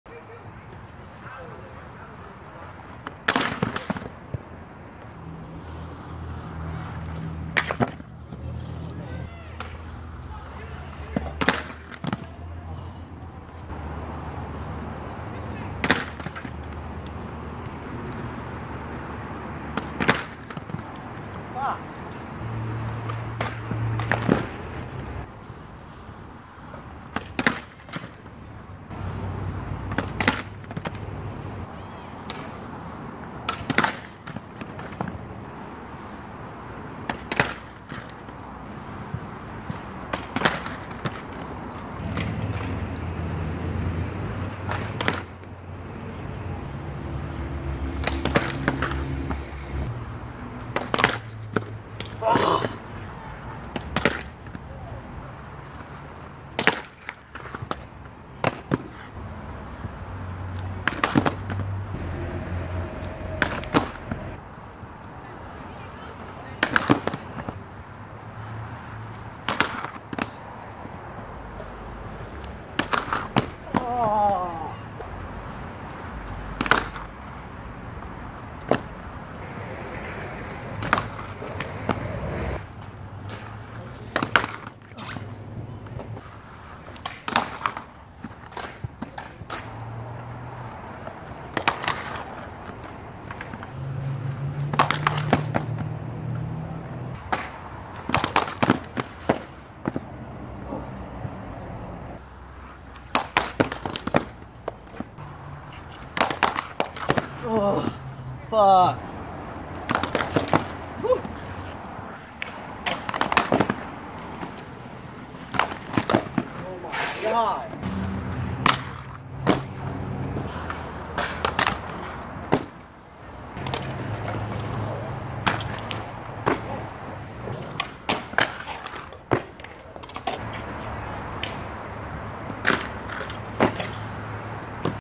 Hard spin soul attempts
Second session attempting hard spin soul